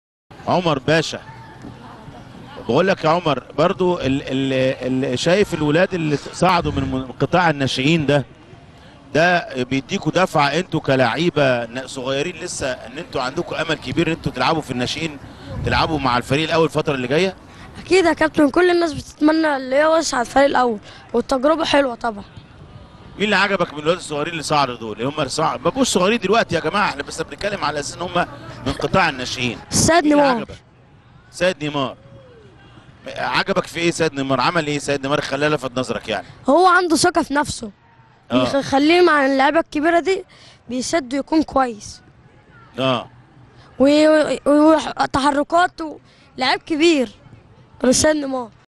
عمر كيلاني: كلنا نريد اللعب في الفريق الأول واللاعبين الصاعدين ممتازين وتجربة ممتازة - ملعب الناشئين
عمر كيلاني: كلنا نُريد اللعب في الفريق الأول واللاعبين الصاعدين ممتازين وتجربة ممتازة - ملعب الناشئين